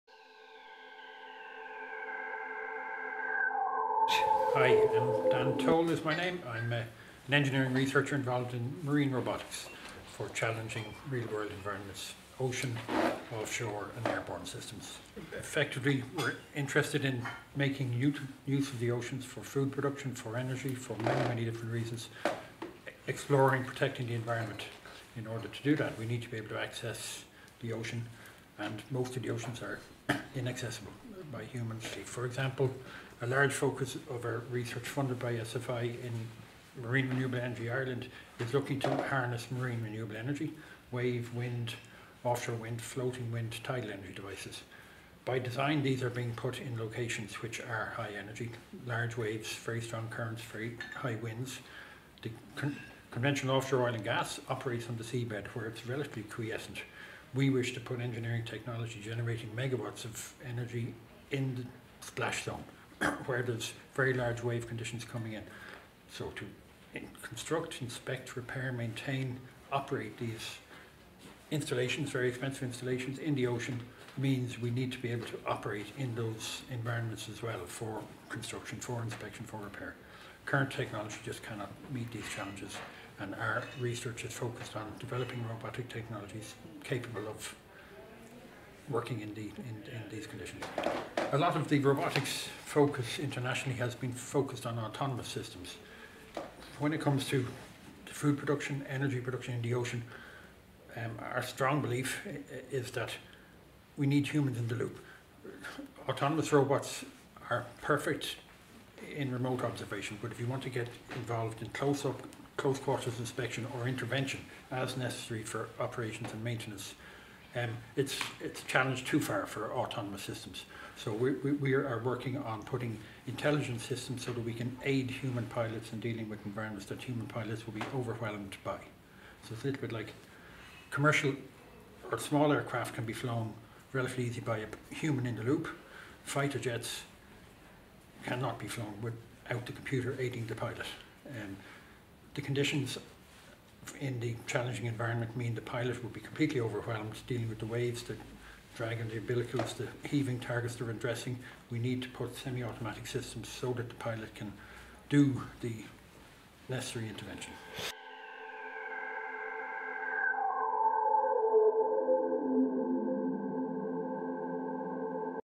Daniel Toal CRIS
Daniel Toal, director of Centre of Robotics and Intelligent Systems, talking about research interests and focus of CRIS.